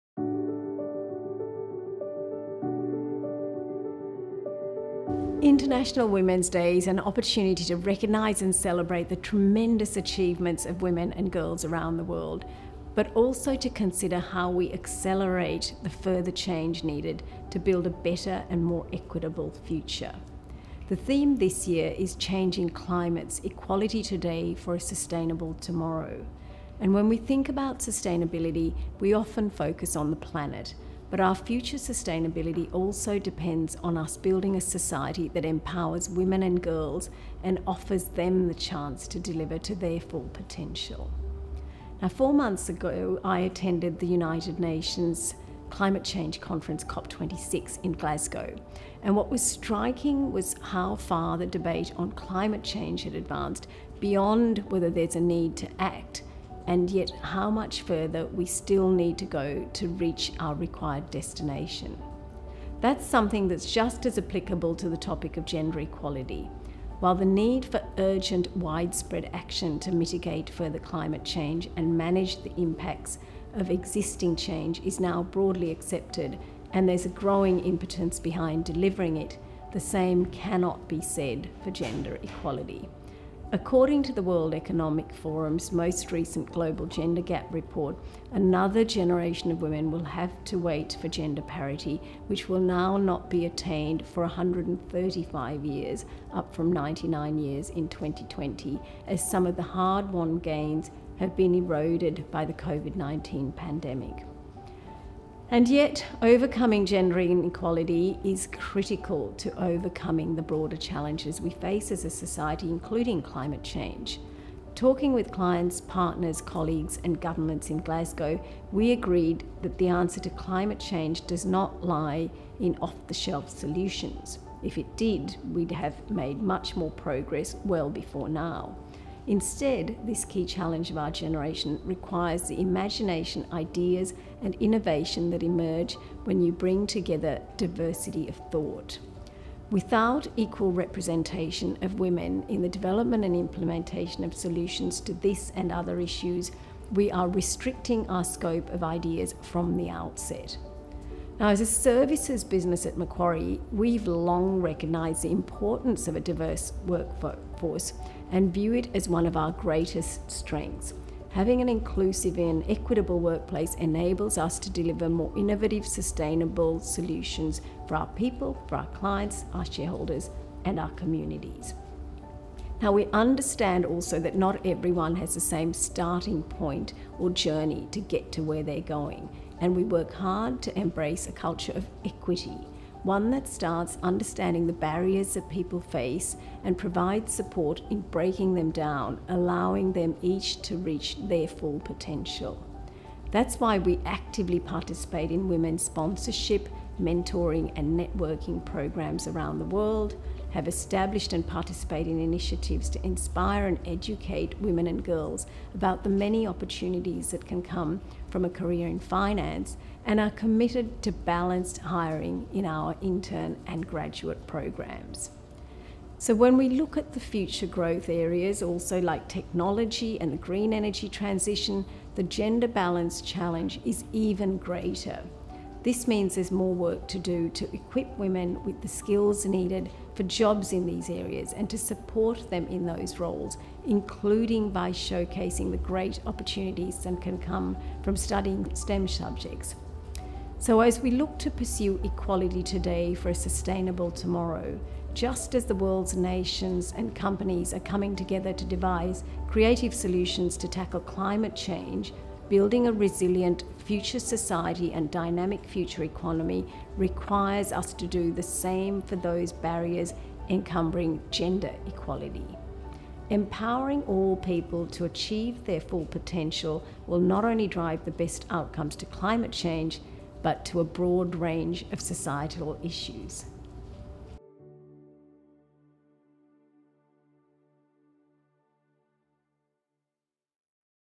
International Women's Day 2022 | Macquarie Group
This International Women’s Day, our CEO Shemara Wikramanayake talks about how our future sustainability depends on us building a society that empowers women and girls and offers them the chance to deliver to their full potential. One that enables us to innovate and respond to a broad range of societal challenges with the benefits that diversity brings.